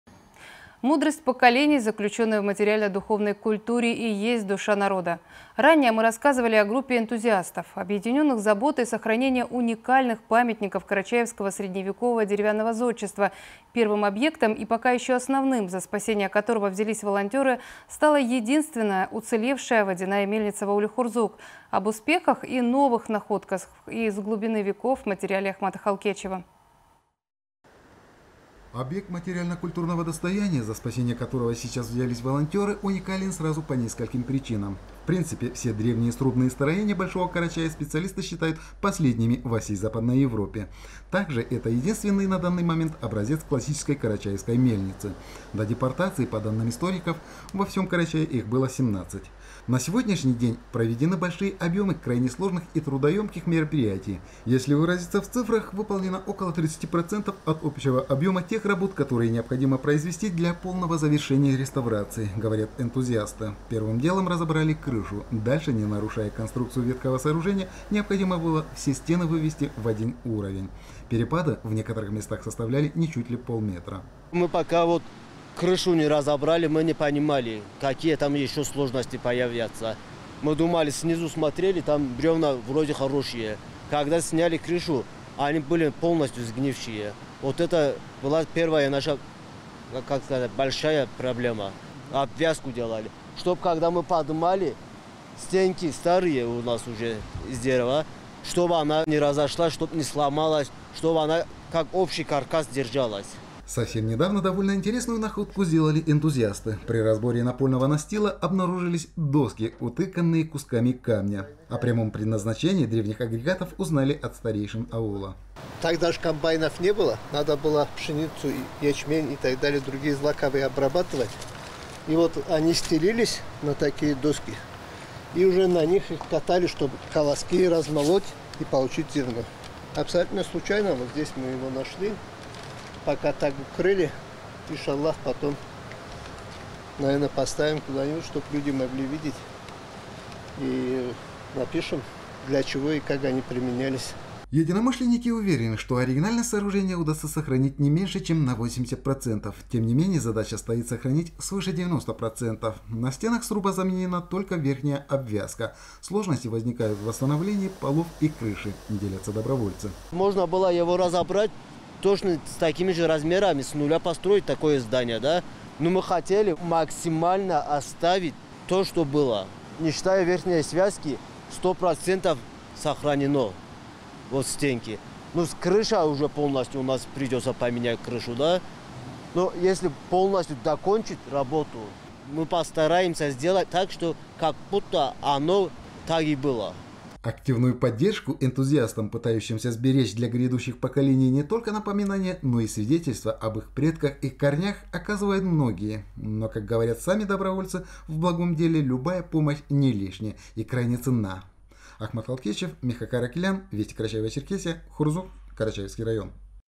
В ауле Хурзук проводятся работы по спасению исторического памятника - водяной мельницы
Мудрость поколений, заключенная в материально-духовной культуре и есть душа народа. Ранее мы рассказывали о группе энтузиастов, объединенных заботой о сохранении уникальных памятников карачаевского средневекового деревянного зодчества. Первым, и пока еще основным объектом, за спасение которого взялись волонтеры, стала единственная уцелевшая водяная мельница в ауле Хурзук.